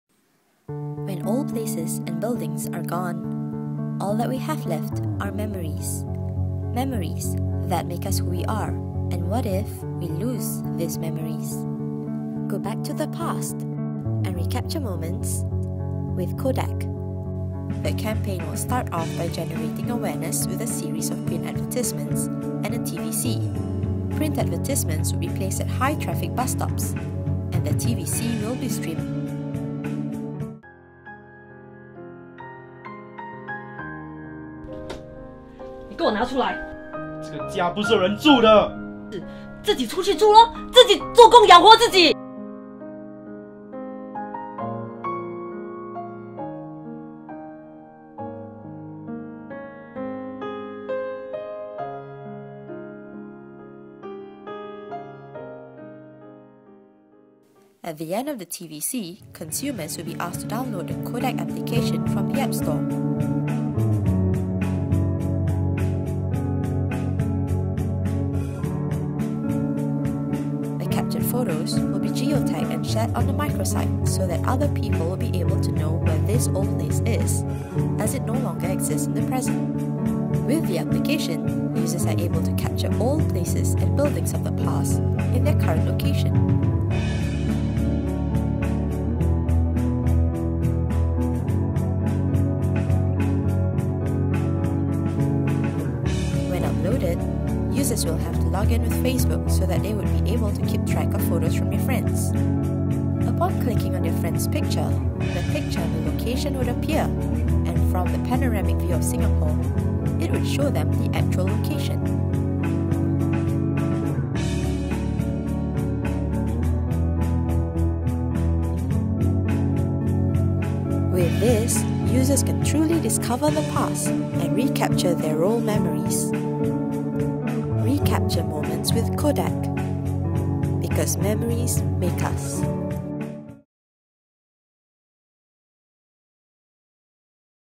Kodak Presentation Video
Kodak memories and people are important to us. It makes us who we are.
Memories are treasures. Once lost, gone forever.
My Final Year Project in LASALLE College of the Arts.
This video is not for commercial use.